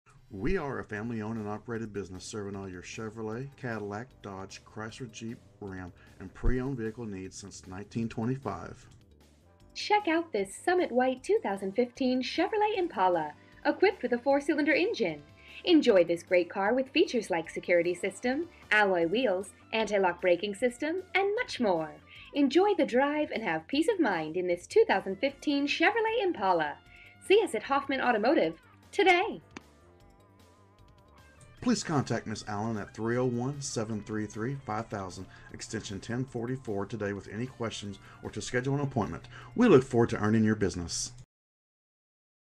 2015 Chevrolet Impala Hoffman Automotive Hagerstown, MD 21740
Chevrolet Impala Imagine driving this Summit White Chevrolet Impala Sedan, equipped with a 4 Cylinder engine. Enjoy this great car with features like Power Driver's Seat, Cruise Control, Tinted Glass, Driver Side Air Bag, Passenger's Front Airbag, Security System, Anti-Lock Braking System, Alloy Wheels, Remote Trunk Lid, Vanity Mirror, Day Night Lever, Keyless Entry, and much more. Enjoy the drive and have peace of mind in this Chevrolet Impala. See us at Hoffman Automotive today!
MILES:     17
VIN:2G1115SL4F9107338

Hoffman Automotive
101 South Edgewood Drive
Hagerstown, Maryland 21740